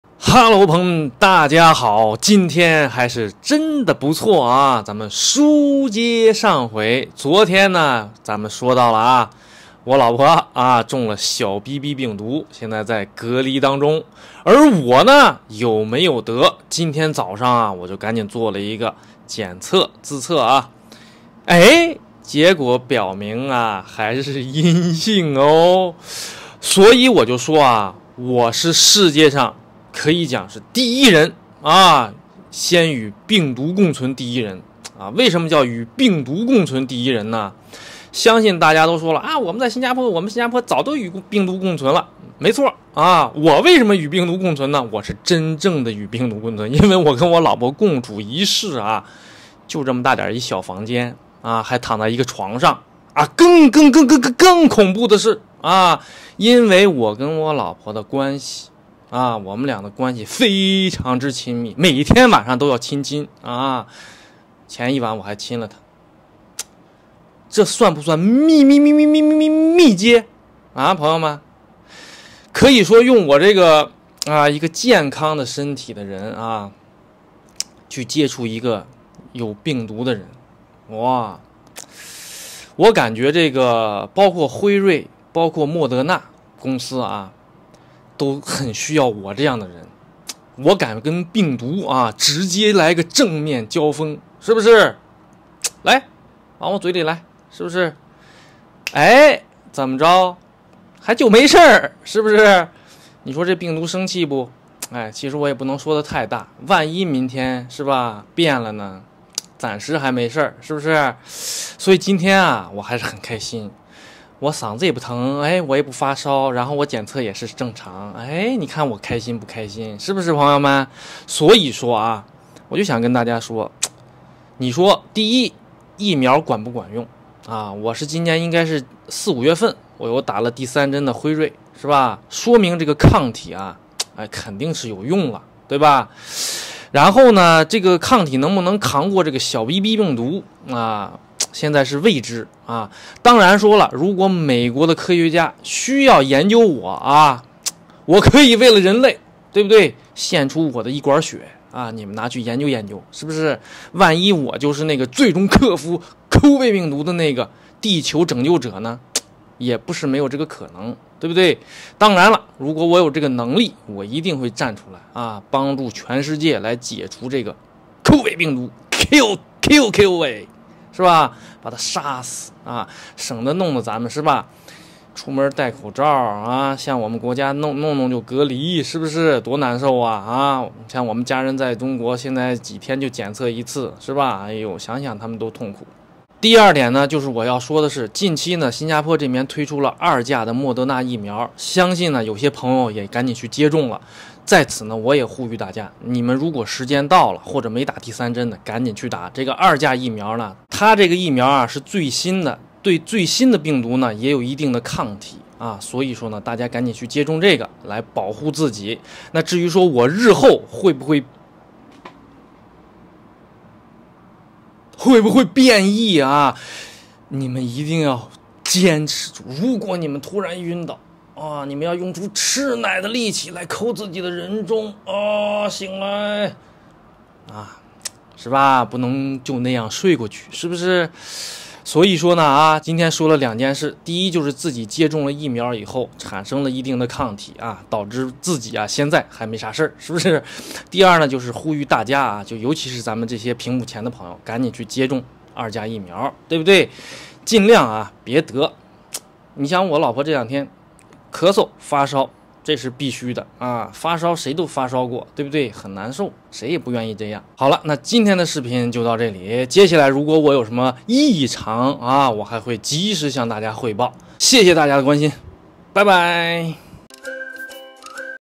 在新加坡与XBB变种病毒密接，是什么样的一种感觉？全球与病毒共存第一人告诉你答案！
What is it like to be in close contact with the XBB variant virus in Singapore? The first person in the world to coexist with the virus tells you the answer!